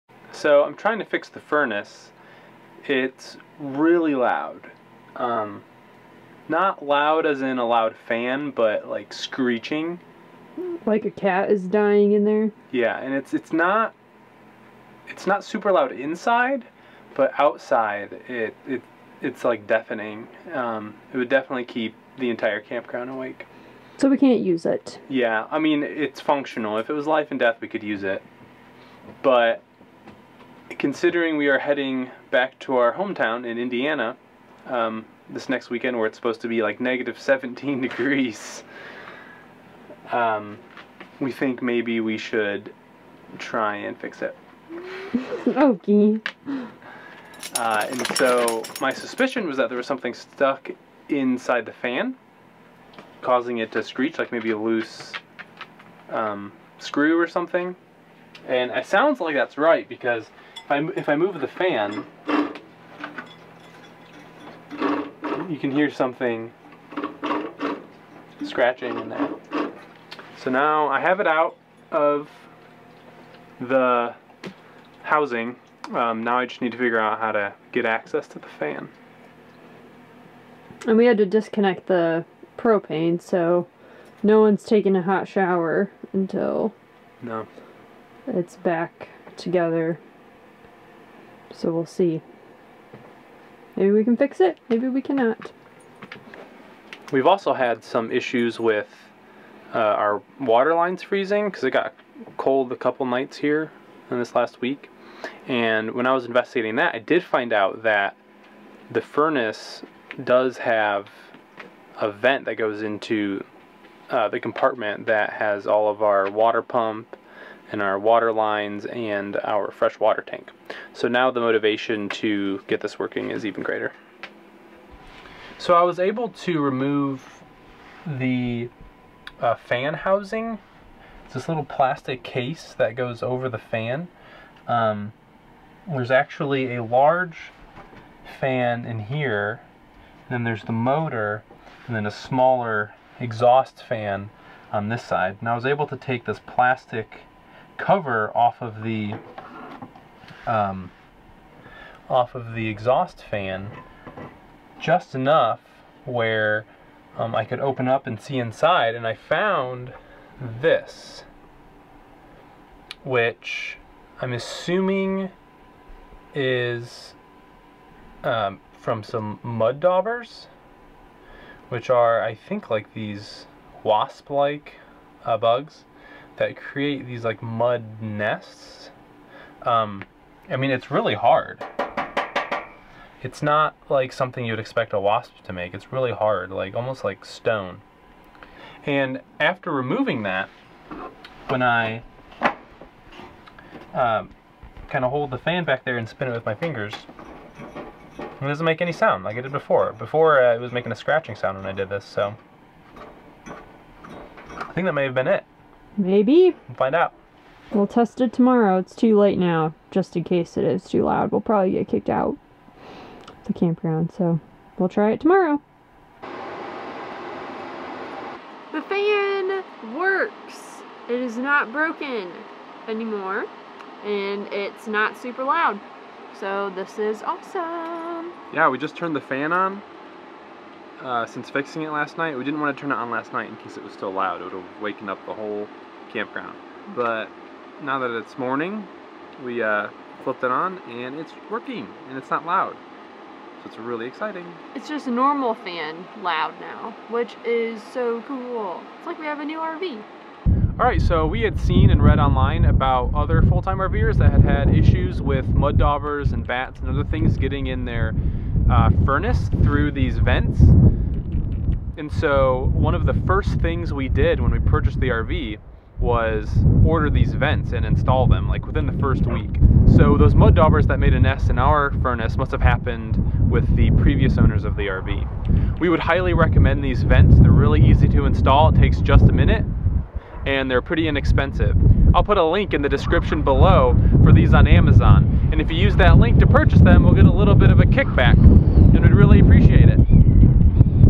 RV Living | Fixing a Loud RV Furnace
The furnace in our RV was making a loud screeching sound, so we fixed it!